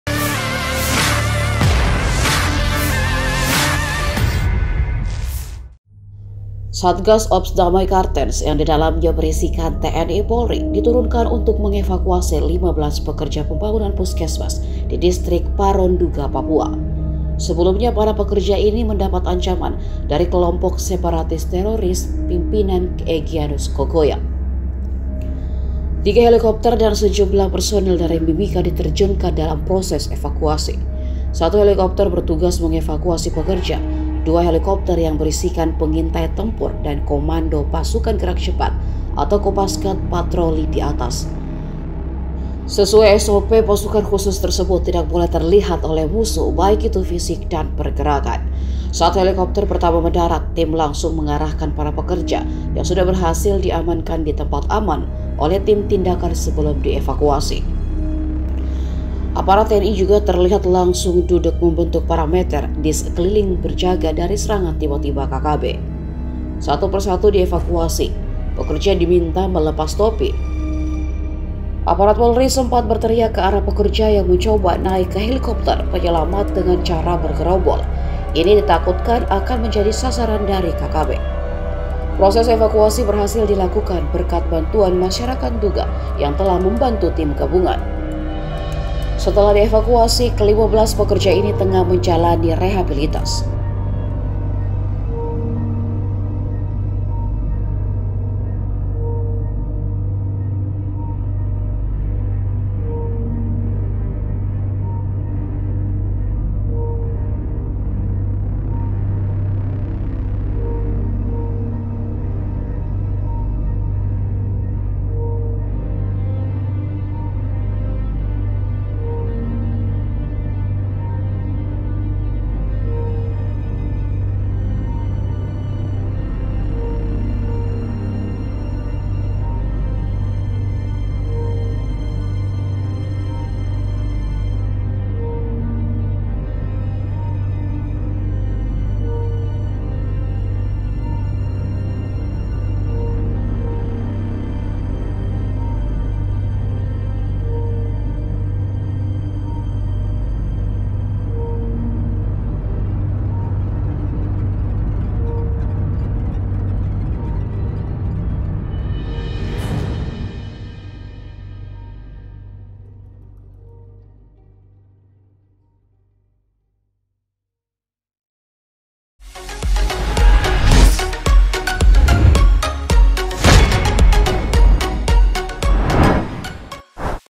Detik detik 15 Pekerja di Nduga Papua Berhasil Dievakuasi TNI Polri
TRIBUN-TIMUR.COM - Satgas Ops Damai Cartenz yang didalamnya berisikan TNI-Polri diturunkan untuk mengevakuasi 15 pekerja pembangunan puskemas di Distrik Paro, Nduga,Papua.

Sebelumnya para pekerja ini mendapat ancaman dari Kelompok Separatis Terotis (KST) pimpinan Egianus Kogoya.

Tiga helikopter dan sejumlah personel dari Mimika diterjunkan dalam proses evakuasi.

Satu helikopter bertugas mengevakuasi pekerja, dua helikopter yang berisikan pengintai tempur dan komando pasukan gerak cepat atau Kopasgat patroli di atas.

Seseuai SOP pasukan khusus tersebut tidak boleh terlihat oleh musuh baik itu fisik dan pergerakan.

Saat helikopter pertama mendarat, tim langsung mengarahkan para pekerja yang sudah berhasil diamankan ditempat aman oleh tim tindakan sebelum dievakuasi.

Aparat TNI juga terlihat langsung duduk membentuk parameter di sekeliling berjaga dari serangan tiba-tiba KKB.

Satu persatu dievakuasi. Pekerja diminta melepas topi. Aparat Polri sempat berteriak ke arah pekerja yang mencoba naik ke helikopter penyelamat dengan cara bergerombol. Ini ditakutkan akan menjadi sasaran dari KKB.

Proses evakuasi berhasil dilakukan berkat bantuan masyarakat Nduga yang telah membantu tim Gabungan.

Setelah dievakuasi ke 15 pekerja ini tengah menjalani rehabilitas.

Narator: Wa Ode Nurmin
Video editor: Hasma Qadri
Sumber: IG Info Komando